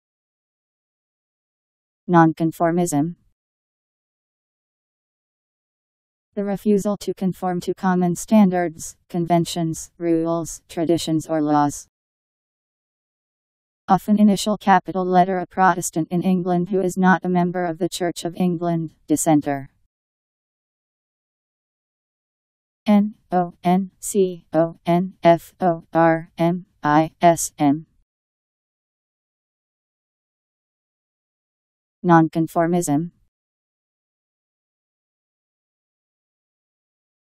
Nonconformism Meaning
Video shows what nonconformism means. The refusal to conform to common standards, conventions, rules, traditions or laws.. (often initial capital letter) a Protestant in England who is not a member of the Church of England, dissenter..  Nonconformism Meaning. How to pronounce, definition audio dictionary. How to say nonconformism. Powered by MaryTTS, Wiktionary